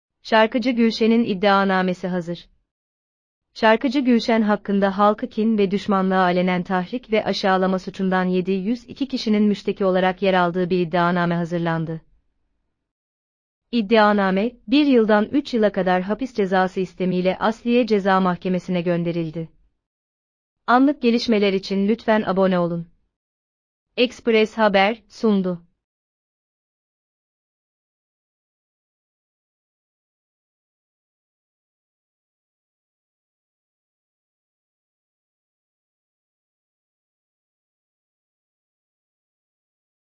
Son dakika... Şarkıcı Gülşen’in iddianamesi hazır
Şarkıcı Gülşen hakkında halkı kin ve düşmanlığa alenen tahrik ve aşağılama suçundan 702 kişinin müşteki olarak yer aldığı bir iddianame hazırlandı. İddianame, 1 yıldan 3 yıla kadar hapis cezası istemiyle Asliye Ceza Mahkemesine gönderildi.